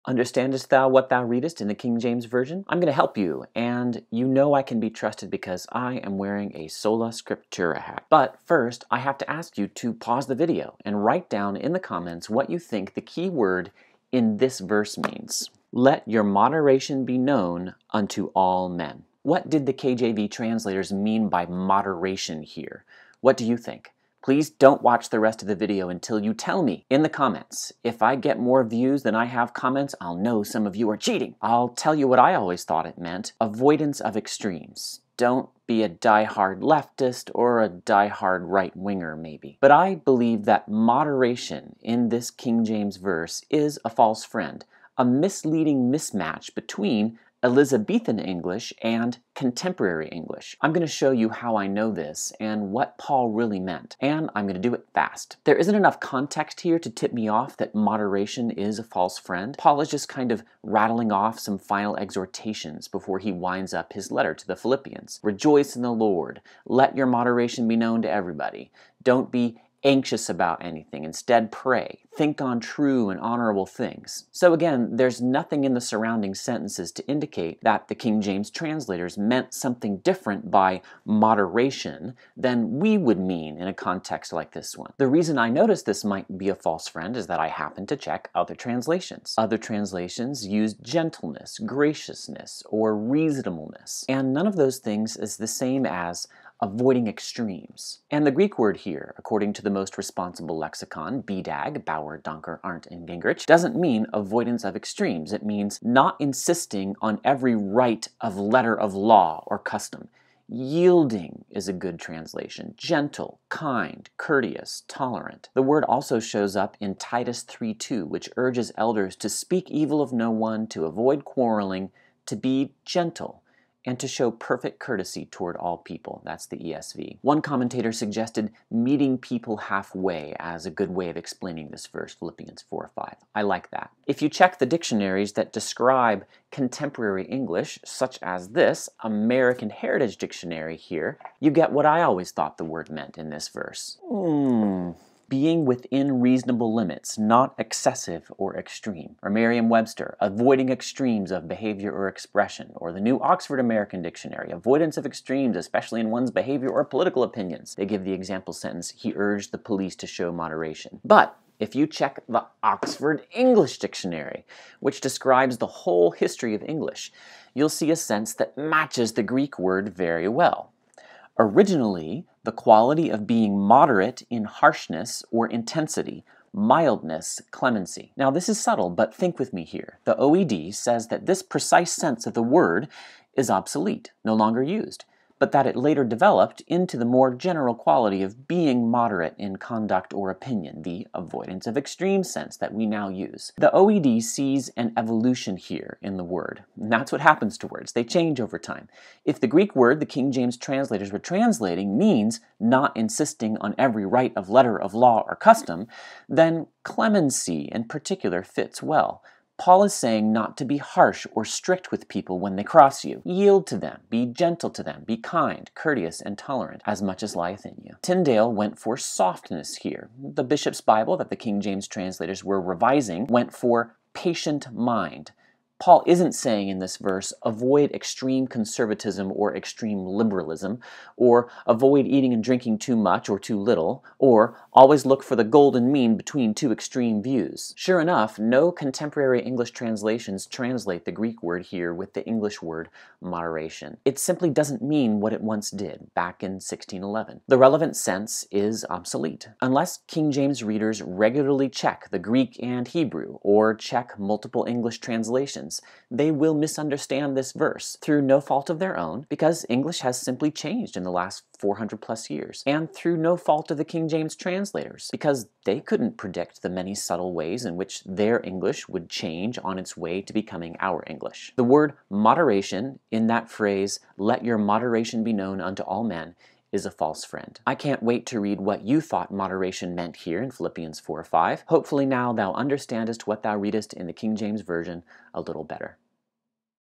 "Let Your Moderation Be Known to All Men" Doesn't Mean What You Think It Means
*Before you watch this video*—write down what you think "moderation" means in Phil 4:5 in the KJV: “Let your moderation be known unto all men. The Lord is at hand.”